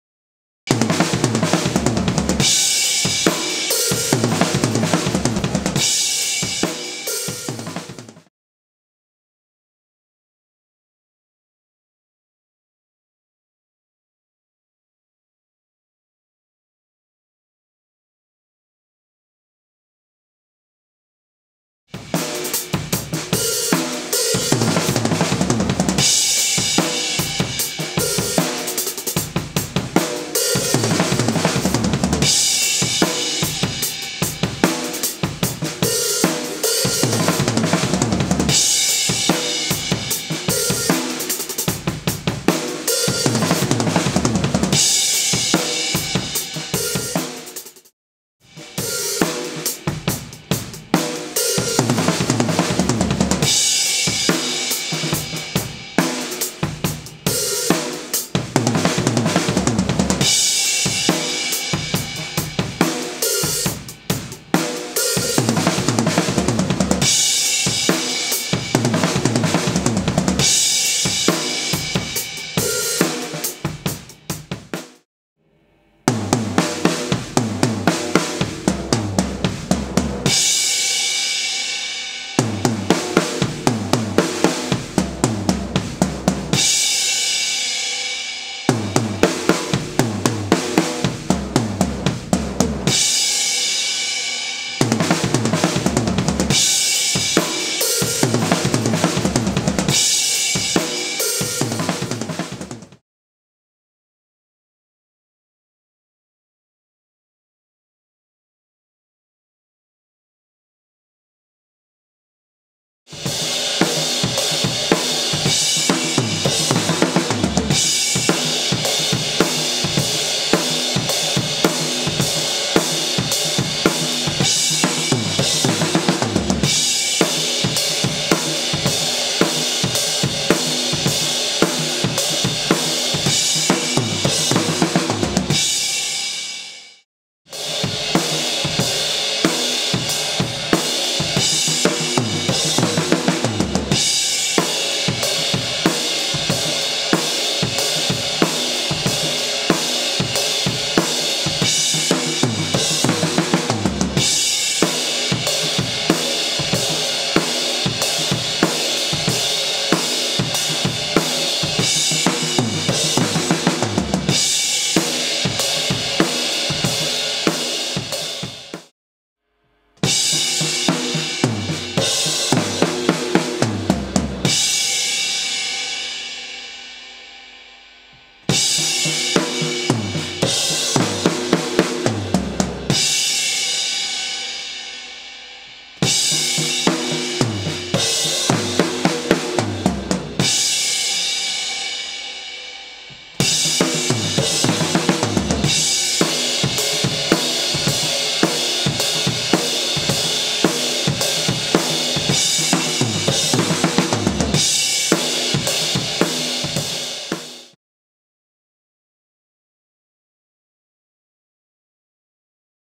Advanced Rock Fills - Drum Lesson 1
In this drum lesson I'll explain 2 different rock fills.

To make it easy for you, I play each fill in 3 tempi: Actual speed, slower and ultra slow. As well as that, you can read the notes in the right upper corner.